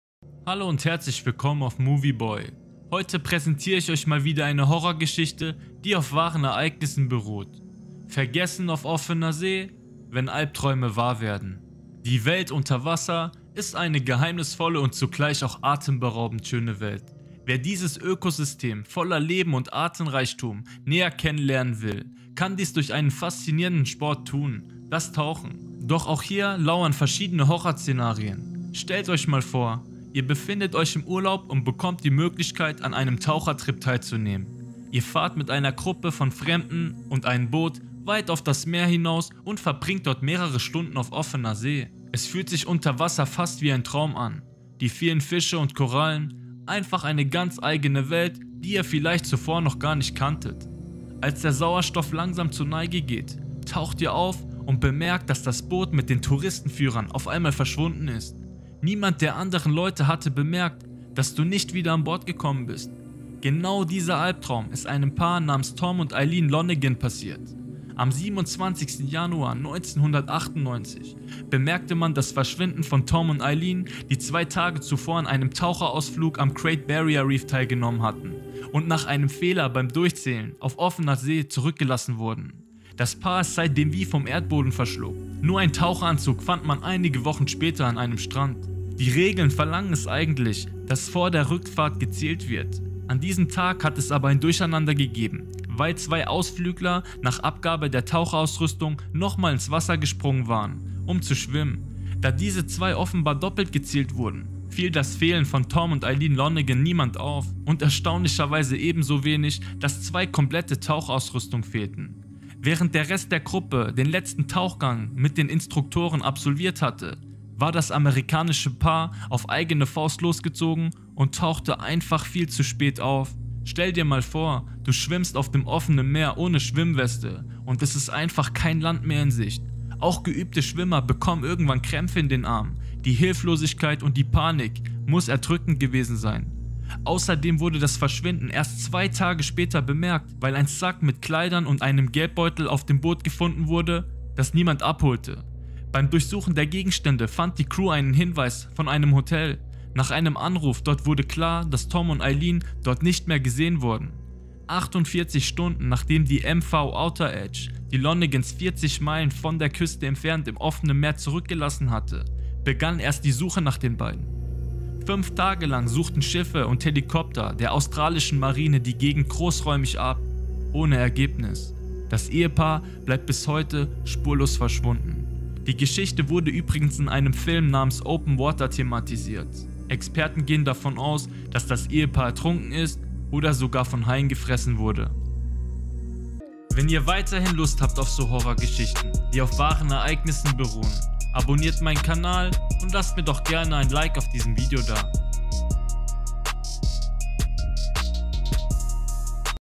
Open Water | Ehepaar beim tauchen im Meer verschwunden (Dokumentation)
Das Verschwinden des Ehepaars Lonergan.
Ermittler gehen davon aus, das der Tod von Tom und Eileen unfassbar schrecklich gewesen sein muss. Doch sind die beiden wirklich ...?
Geschichte wird in dem Film Open Water aufgearbeitet.